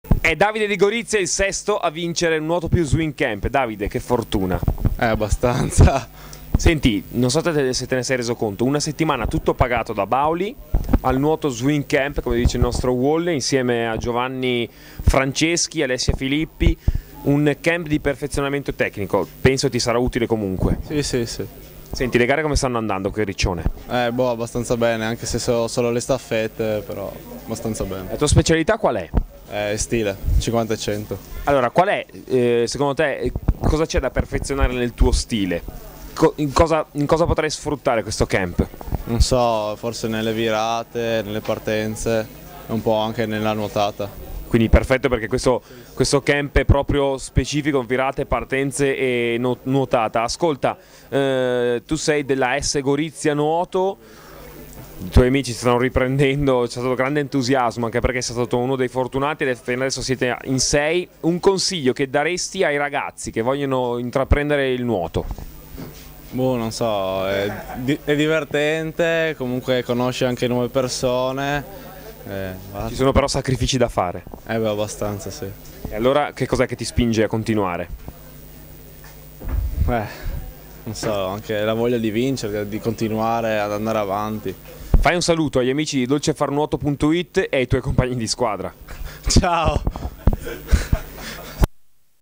La tua intervista da campione - Davide da Gorizia, sesto vincitore del concorso offerto da Bauli
Davide da Gorizia, sesto vincitore del concorso "Vinci un Nuoto+SwimCamp", offerto da Bauli e Dolce far nuoto durante i Campionati di Riccione 2011.